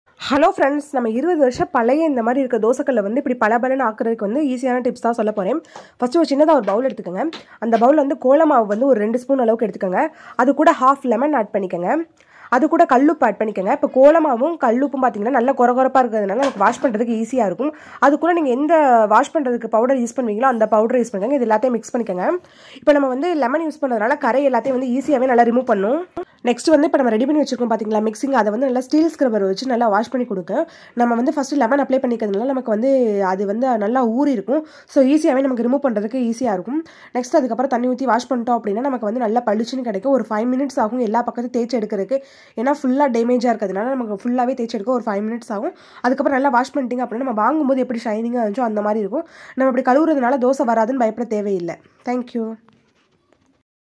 1 நிமிடத்தில் தோசைக்கல் புதுசு போல் மின்ன இப்படி செஞ்சு பாருங்க
கேஸை மிச்சபடுத்த செம்ம ஐடியாவா  இருக்கே//வருடத்திற்கு 2 சிலிண்டர் மட்டும் போதும்//how to save LPG gas cylinder

கொசு எப்படி இருக்கும்னு கூட மறந்துருவிங்க இப்படி ஒரு தடவை செஞ்சு பாருங்க//mosquito kill tips